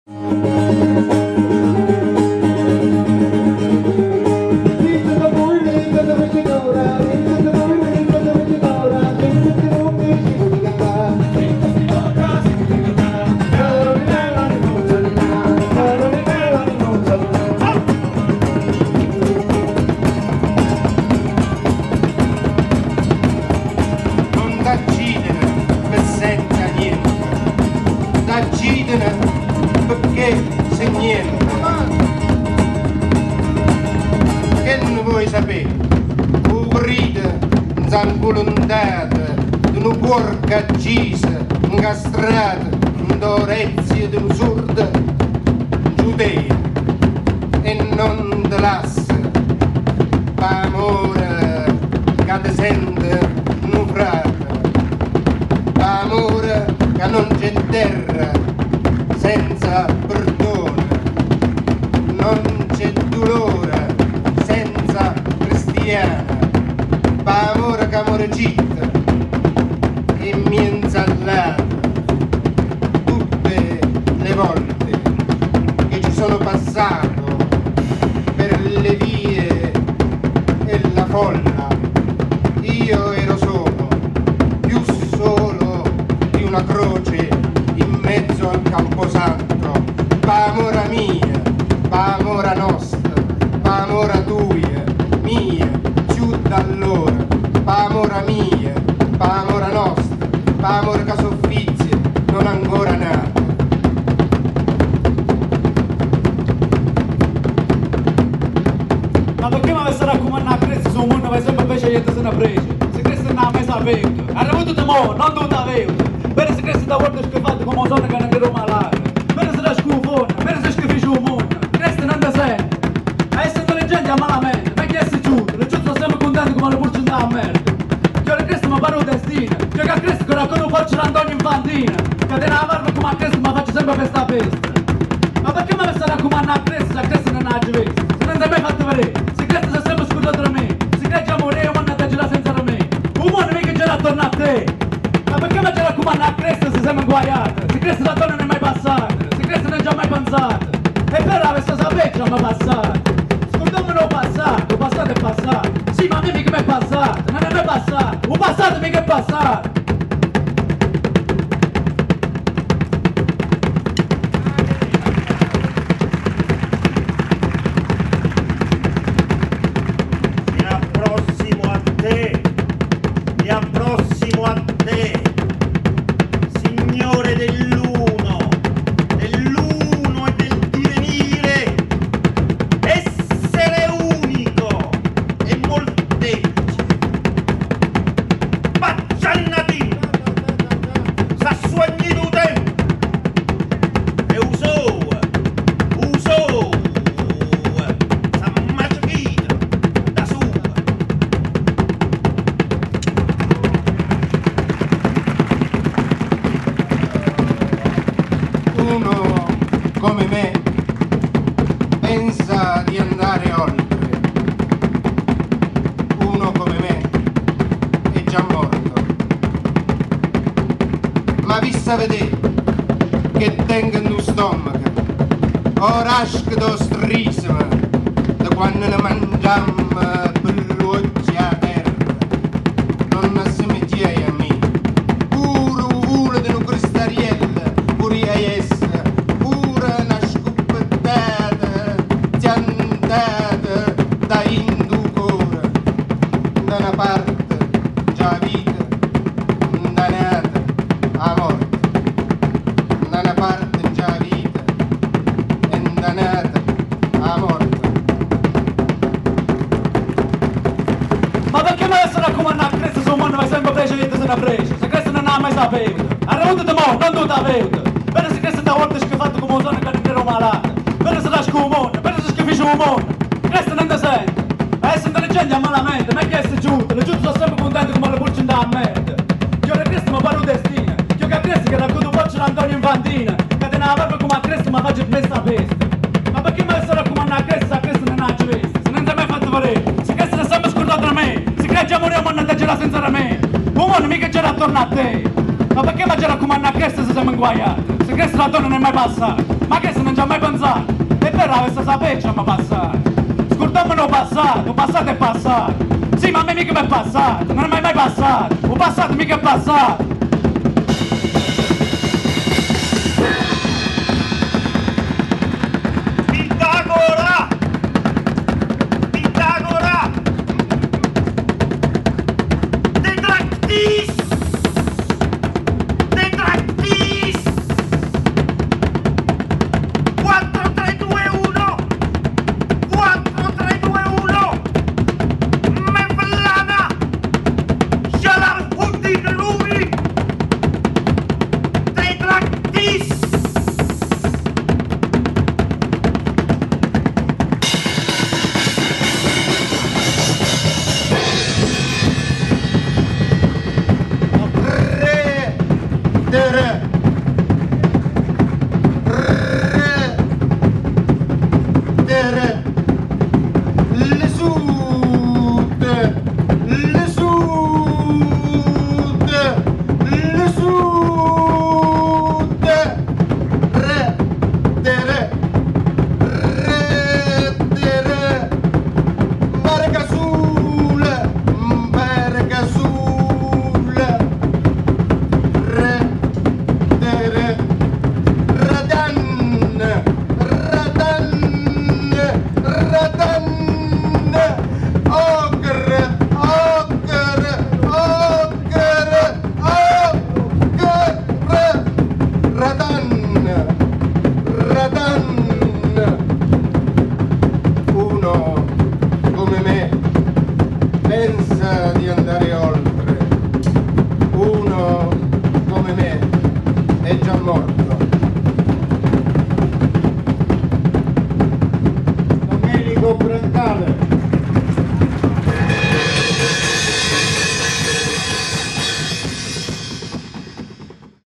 Antonio Infantino con Domenico Brancale - Donato Montesano - Aurelio Giordano
Durante il festival La luna e i calanchi ad agosto 2013 in Aliano - Basilicata una performance straordinaria con il maestro Infantino a musicare le emozionanti parole di tre poeti lucani: Domenico brancale - Donato Montesano - Aurelio Giordano